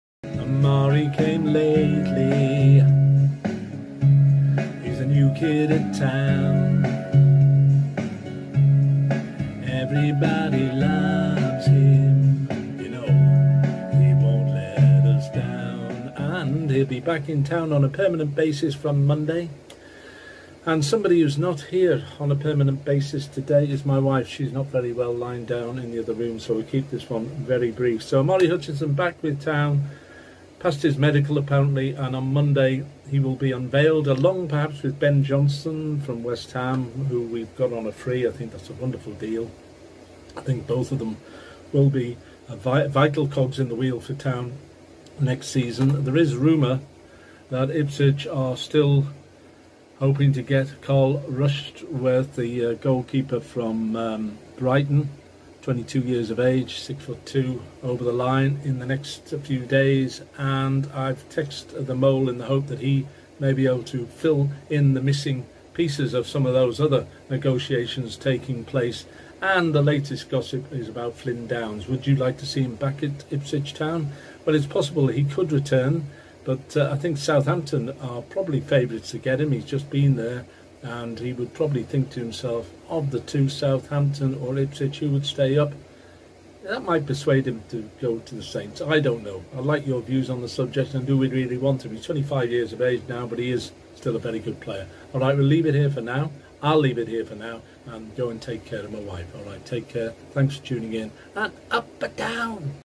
Four Through The Door Next Week at IPSWICH TOWN?!
Well, it could happen because apart from Omari Hutchinson and Ben Johnson joining our ranks, negotiations are in play for  Brighton keeper Carl Rushworth, and perhaps too, Flynne Downes and these deals might materialise in the coming days.